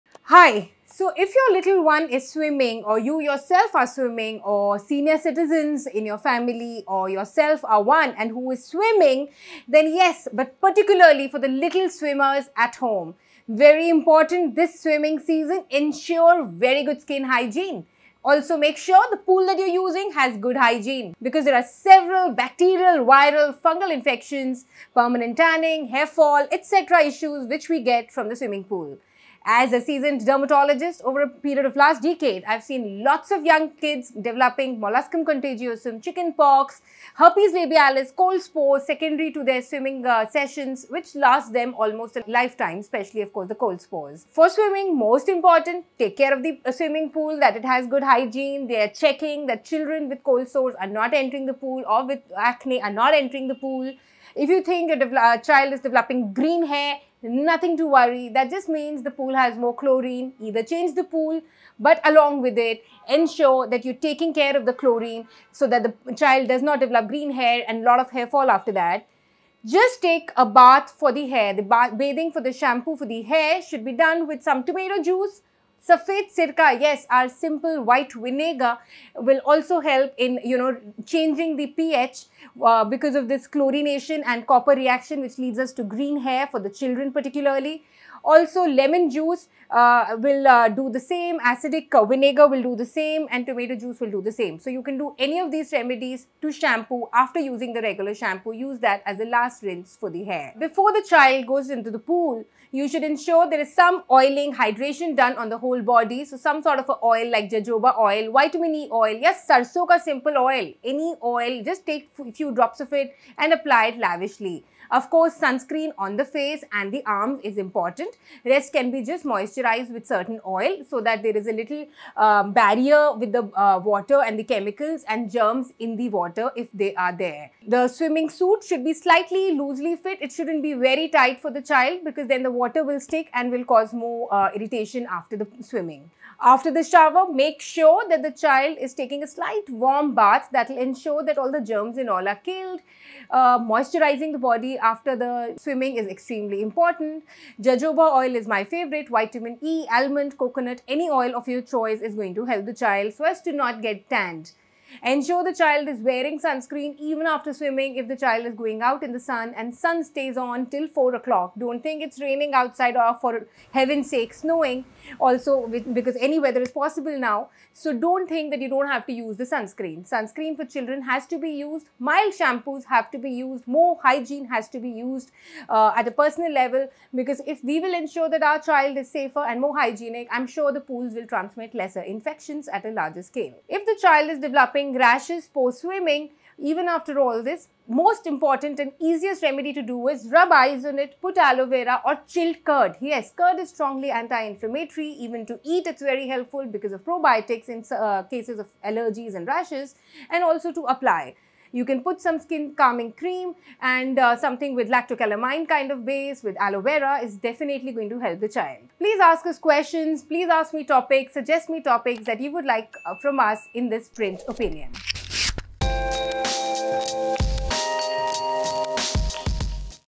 Side effects of swimming & how to cure them
Swimming, as enjoyable as it is, can lead to rashes, hairfall, and a variety of skin infections. Watch dermatologist Dr Deepali Bhardwaj explain the types of ailments that can occur as a result of swimming, and what you can do to prevent and cure them.